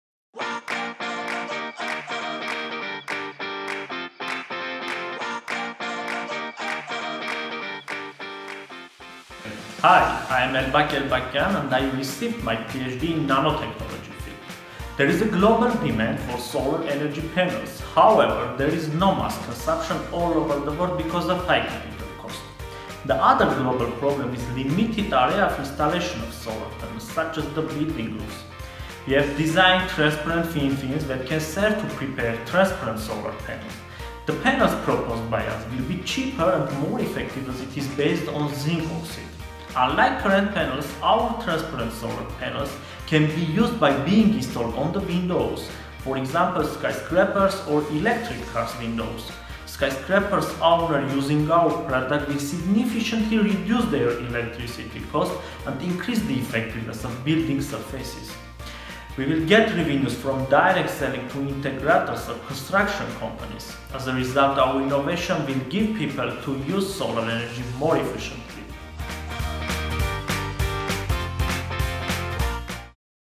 CSBIC2019 Finalists - Transparent Solar Panels
Transparent Solar Panels
• Elbak Elbakyan Araksi Poghosyan Hovhannes Melikyan
• American National Engineering Laboratories